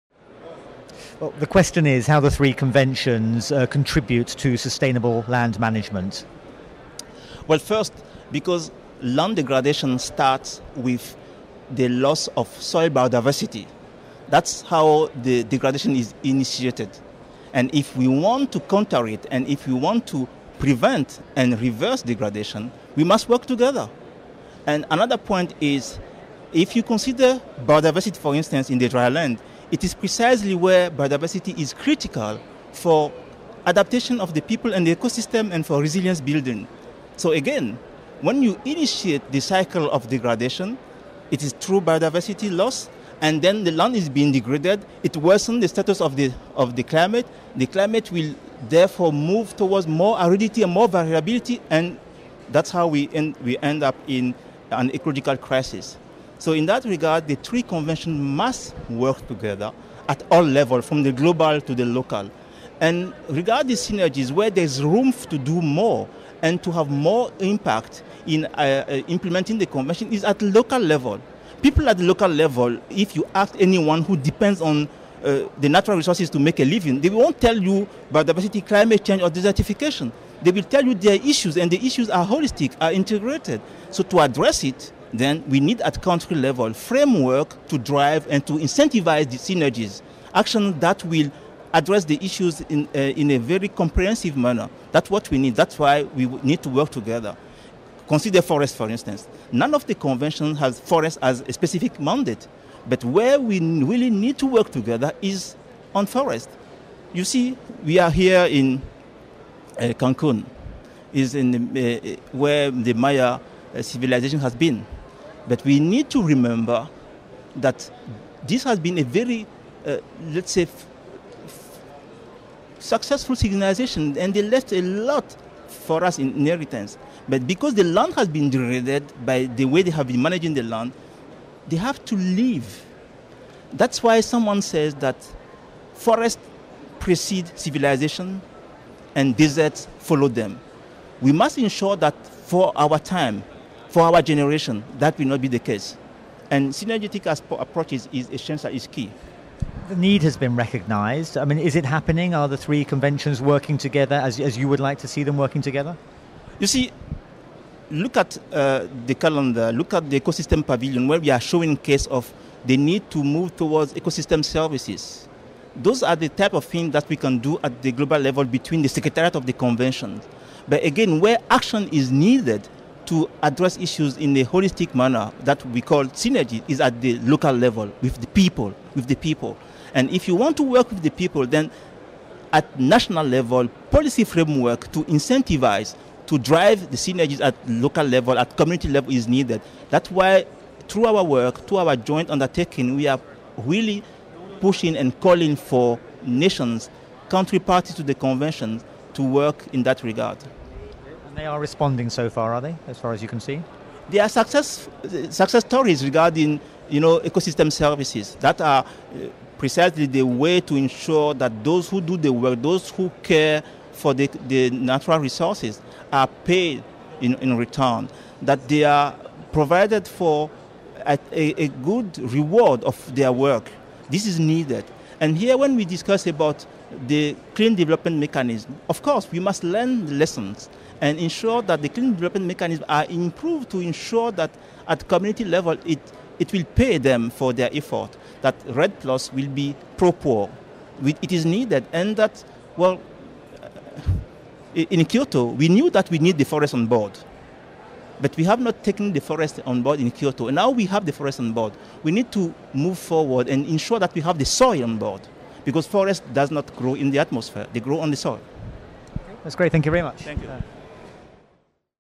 Luc Gnacadja, Executive Secretary of the United Nations Convention to Combat Desertification
Luc Gnacadja, Executive Secretary of the United Nations Convention to Combat Desertification, explains how biodiversity loss leads to degradation of the environment, and how we end up in crisis. He says the three Rio Conventions need to work together, and explores the need to focus on the local level. "The issues are holistic and integrated".